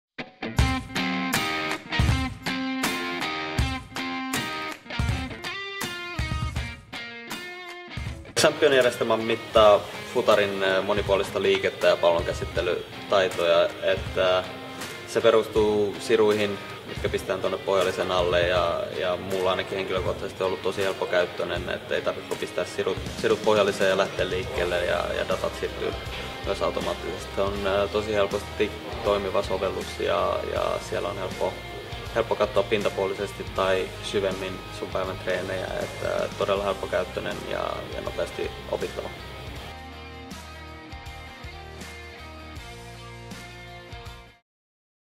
Jere Uronen haastattelussa, osa 1 | Mikä on Xampion?
Suomen maajoukkuemies Jere Uronen on käyttänyt jalkapalloilijan teknisiä ja fyysisiä kykyjä mittaavaa Xampion-järjestelmää useita kuukausia ja kertoo kokemuksiaan.
Mikä Xampion on ja millaista sen käyttö on?